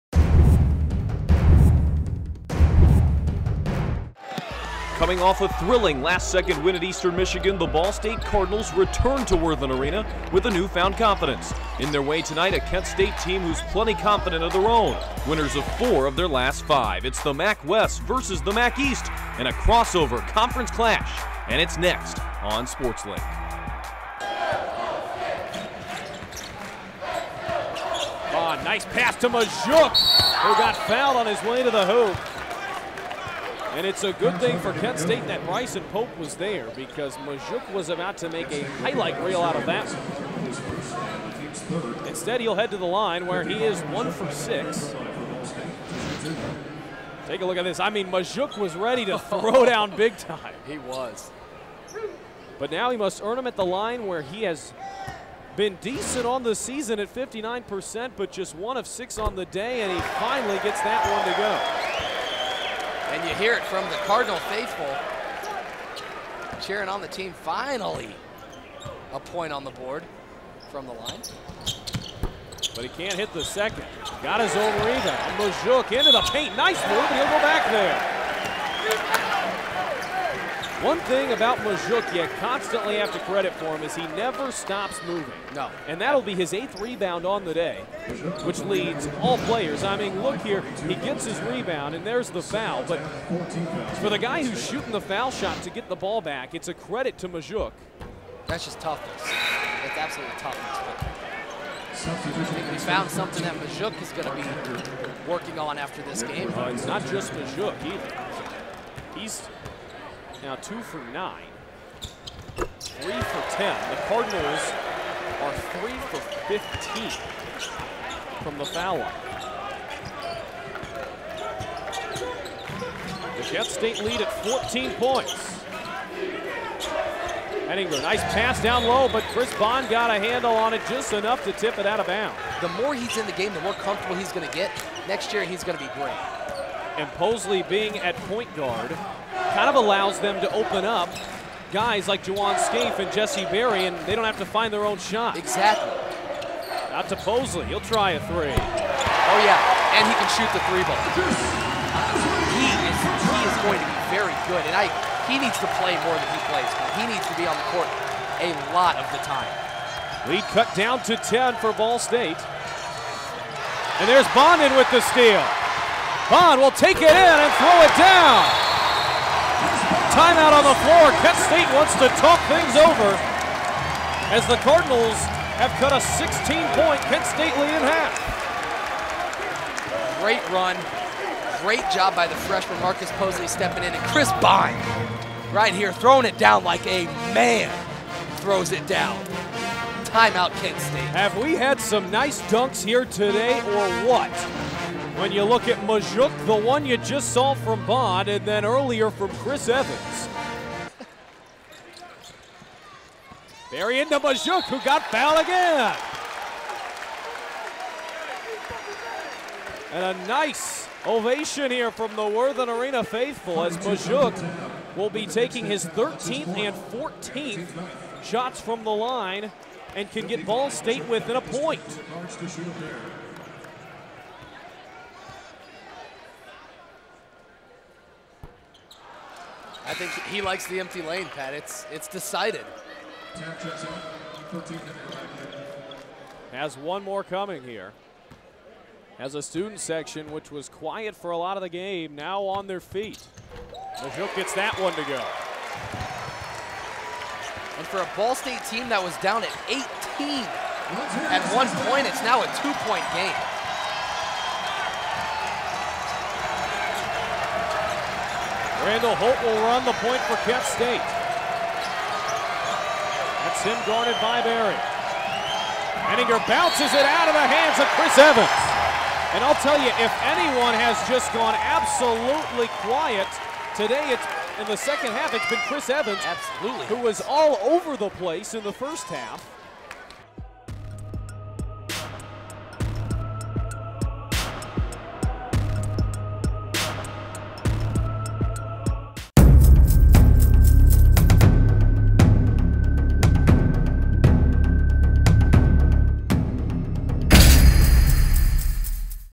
Ball State Sports Link: MBB vs Kent State Broadcast
Ball State Sports Link broadcasted Men's Basketball vs. Kent State on January 12. Here's a look at some of the highlights from the broadcast.